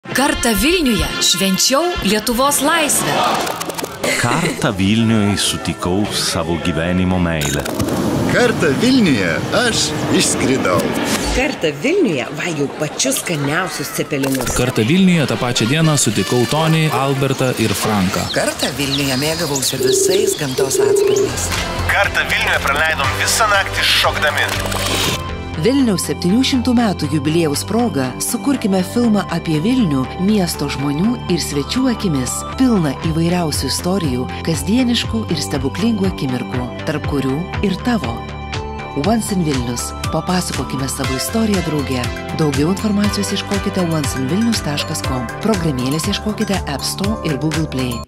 Kartą Vilniuje. Papasakok savo istoriją su „Once in Vilnius“
Mūsų išmanioji programėlė „Once in Vilnius“ kviečia visus – ne tik vilniečius, bet ir miesto svečius – dalintis savo istorijomis, kurios nutiko mūsų visų mylimame Vilniuje. Kelkite vaizdo įrašus, pasakokite ir komentuokite užfiksuotas akimirkas, parodykite ne tik miesto erdves, bet ir įvykius, emocijas ir taip padėkite atskleisti unikalų bei savitą Vilnių.

Iš visos į programėlę įkeltos vaizdo medžiagos „Once Upon a Time in Vilnius“ filmo kūrėjai montažui tikisi atrinkti 700 kadrų. Kiekvienas į programėlę įkeltas kadras pretenduoja tapti filmo dalimi, tad papasakokime savo istoriją drauge.

Our "Once in Vilnius" app invites every one of you - whether you're a Vilnius resident or a guest of the city - to share your stories about our beloved city Vilnius. Upload your videos while sharing and interacting with other users to help us reveal the unique and special essence of Vilnius.

Each photo or video uploaded to the app could be part of our final film "Once Upon a Time in Vilnius". There will be 700 sequences selected for the final montage from all the material uploaded to the app. So we all can tell our story together.